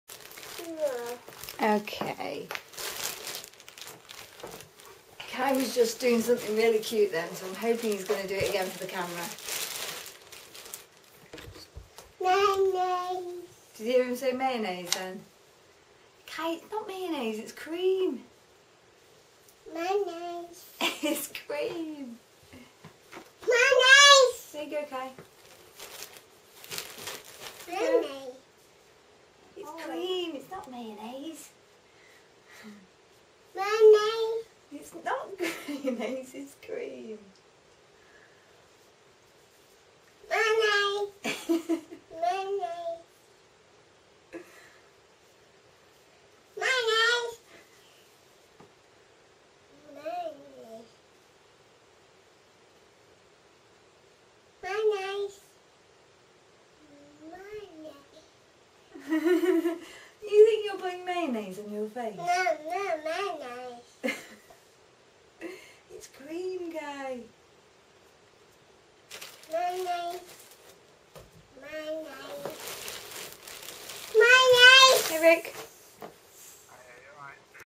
Khai rubs face cream on his face & calls it "mayonnaise"
This is a personal movie for my friends and family only of my little lad.

There is no hilarious thing that happens - i just thought it was cute the way he says "mayonnaise" and then rubs it on his face!